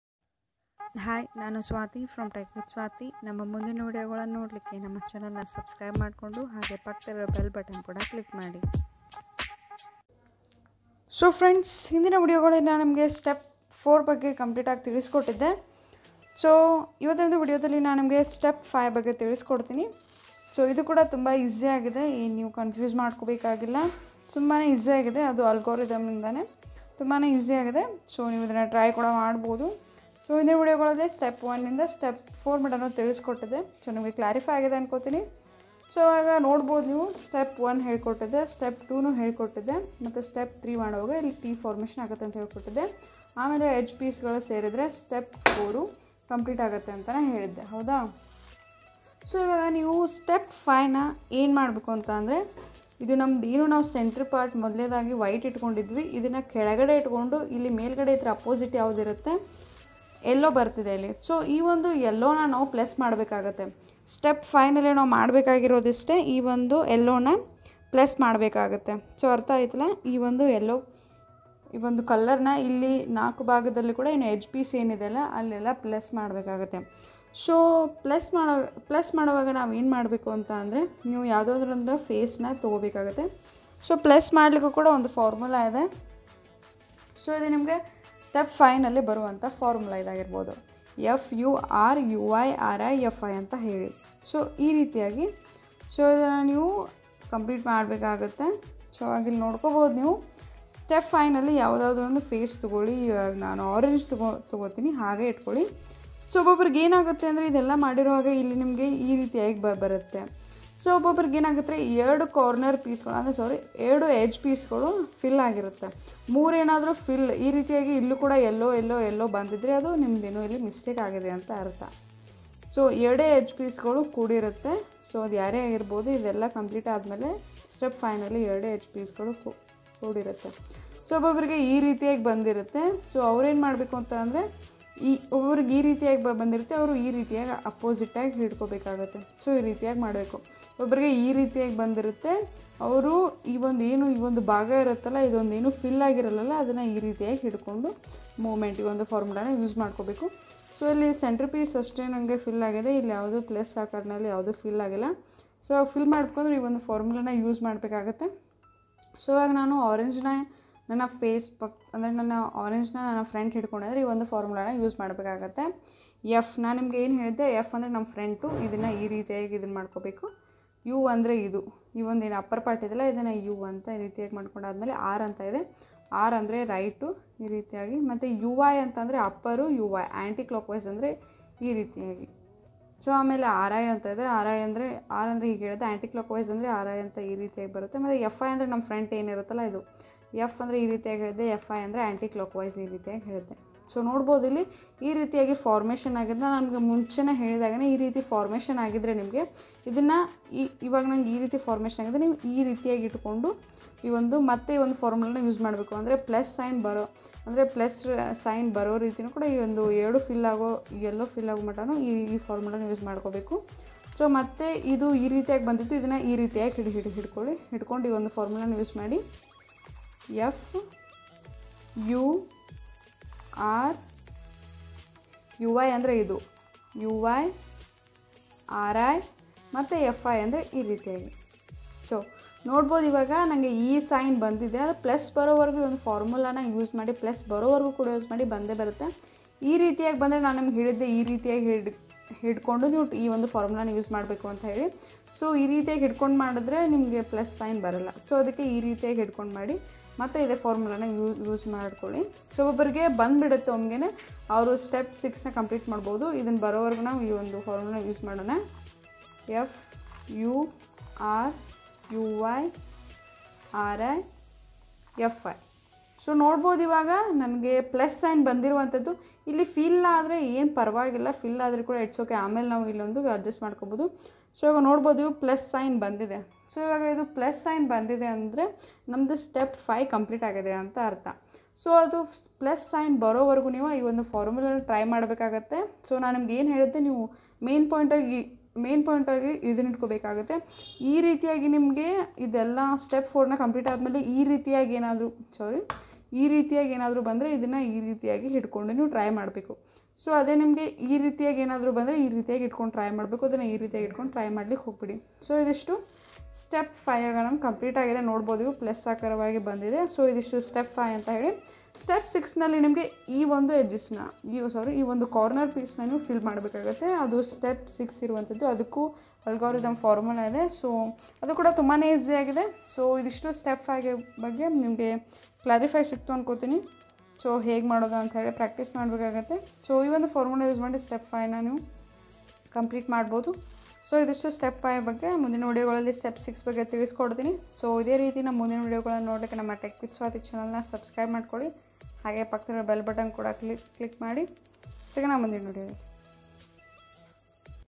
3×3 Rubik's Solve|Part=5|Step=5|EXPLAINED IN KANNADA
3×3 Rubik's Solve|Part=5|Step=5|[ಕನ್ನಡದಲ್ಲಿ] ತುಂಬಾ ಸುಲಭವಾಗಿ ಈ ಒಂದು Rubik's ಹೇಗೆ solve ಮಾಡೋದು ಅಂತ ಈ ಒಂದು ವಿಡಿಯೋದಲ್ಲಿ ತಿಳಿಸಿ ದ್ದೇನೆ|ಮುಂದಿನ steps ಗಳಿಗಾಗಿ wait ಮಾಡಿ|2020